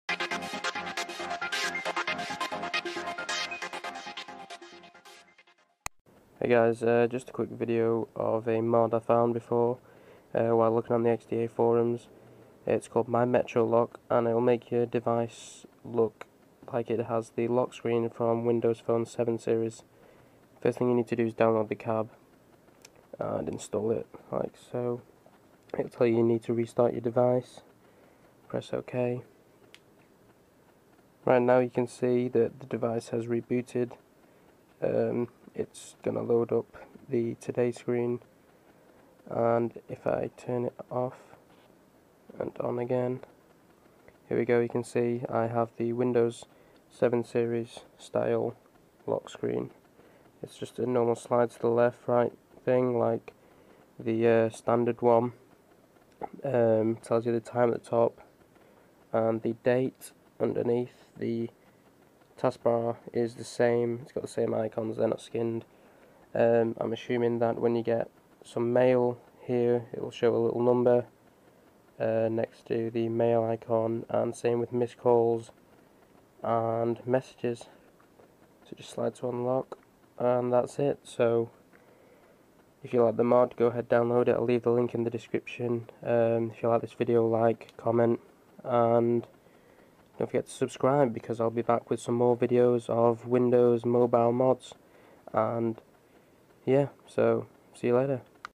Windows Mobile: Windows Phone 7 Metro Lockscreen
This mod makes your lockscreen look like the lockscreen from windows mobile 7